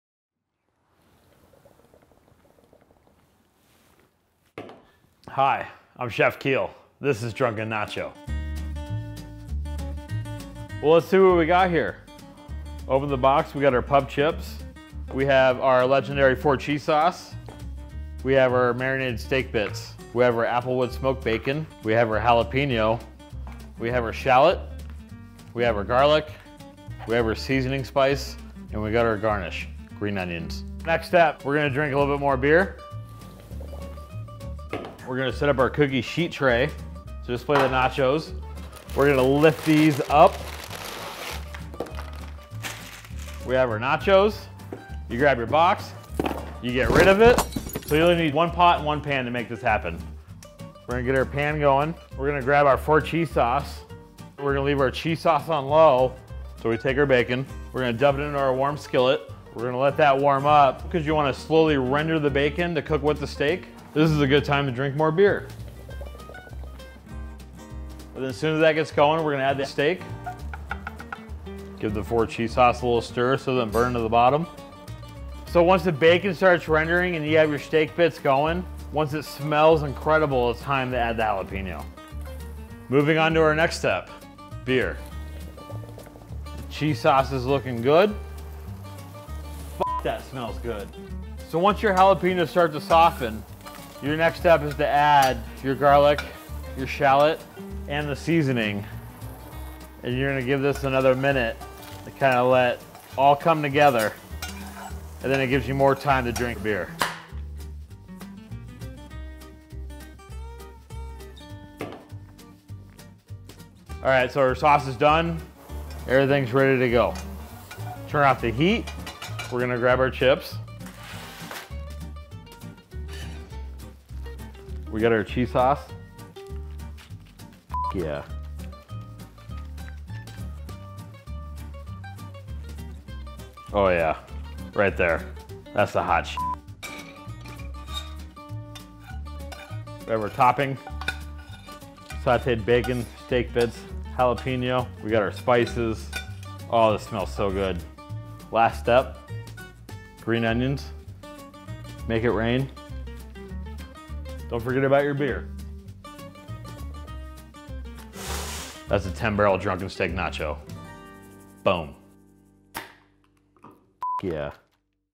Make it at Home: Drunken Nachos
Snag a Nacho Box from our pub, crack a beer or two, and learn how to make 10 Barrel’s infamous steak nachos alongside our Denver Head Chef, Kiel Ard. And don’t forget the beer!

Order our Nacho Boxes from a pub near you